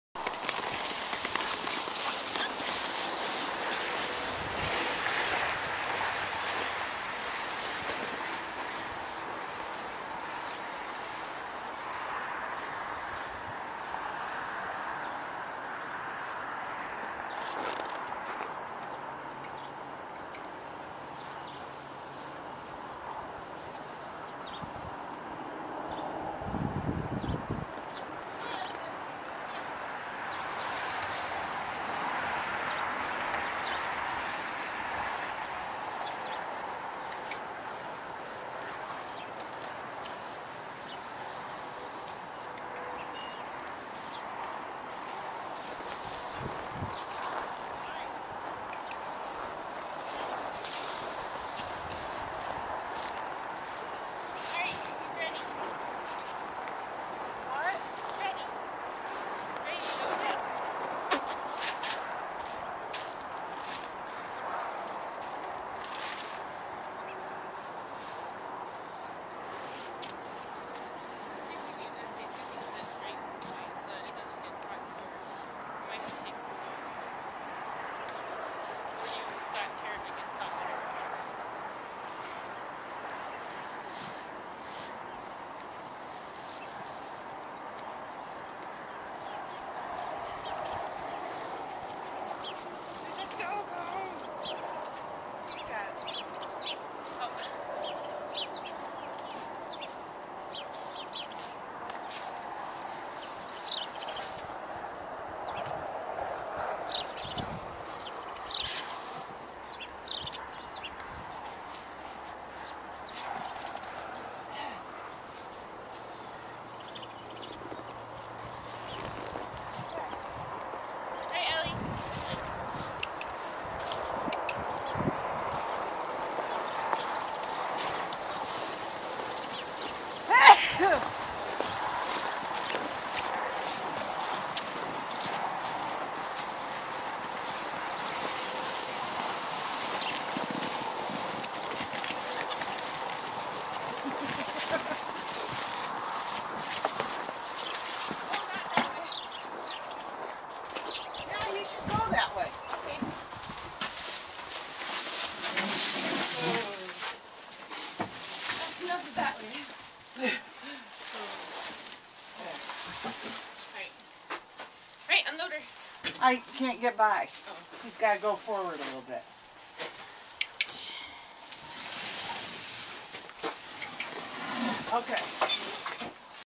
Purposeful Toboggan Pulling
Blu lent us a "hoof" (4 actually) with the straw we picked up early this morning. He even left his breakfast! When I went out to catch him, he touched the halter in my hand as I was greeting him, which is his way of telling me to put it on. He was so accepting of everything. Recorded on January 17, 2011 using a Flip Video camcorder.